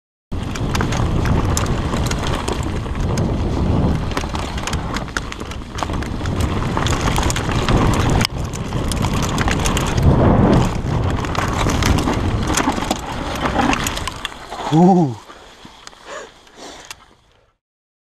!Nochmal gut gegangen! ...FAST Fail *ENDURO 1080p60
Tjo... Viel zu schnell, bissl Quer, dann viel Angst aber alles nochmal gut gegangen. :-D

★Mein Bike★
Propain Tyee

★Kamera★
GoPro Hero 3+ Black Edition